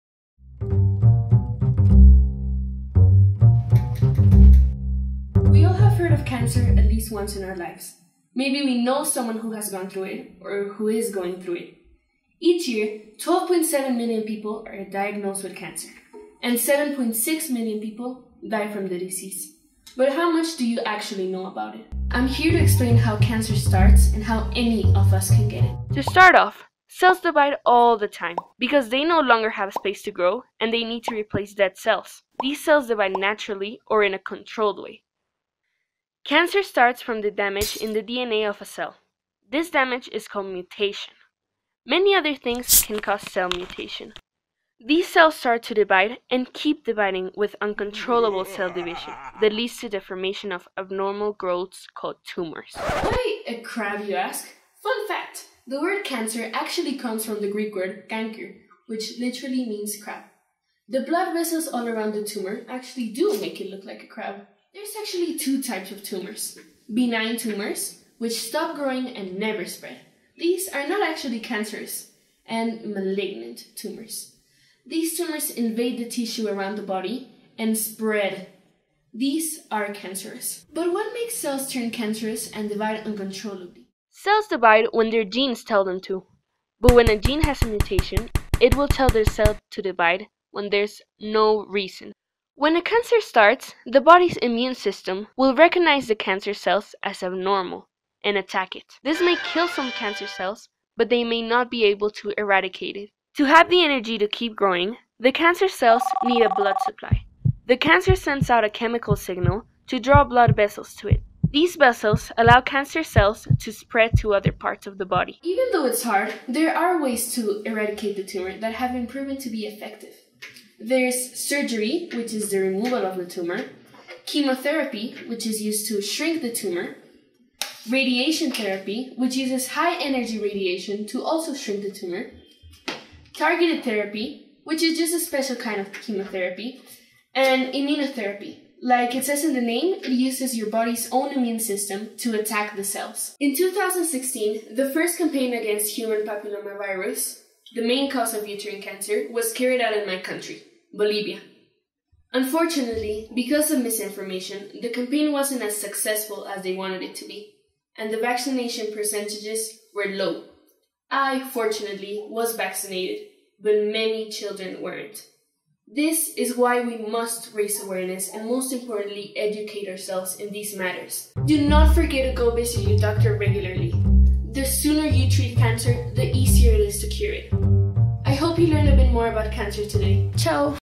Cancer: Breakthrough Junior Challenge 2017
While looking for topics i wanted to touch on something important, something complicated to understand but very important to understand, while researching about cancer and talking to people about it i realized so many people had a misinterpretation about it.
With this video, i want people to understand how cancer comes about and how to protect yourself against.
I hope you learned a lot with this video!